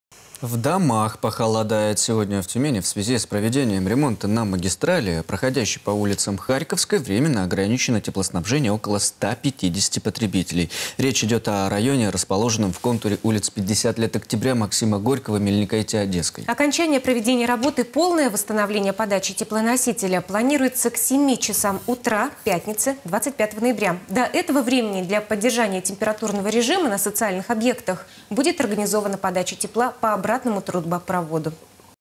В одном из районов Тюмени ограничено теплоснабжение
В Тюмени ограничено теплоснабжение в районе, расположенном в контуре улиц: 50 Лет Октября - М. Горького - Мельникайте - Одесская. Это связано с проведением ремонтных работ. Их окончание планируется к 7 часам утра 25 ноября. До этого времени для поддержания температурного режима на социальных объектах, будет организована подача тепла по обратному трубопроводу.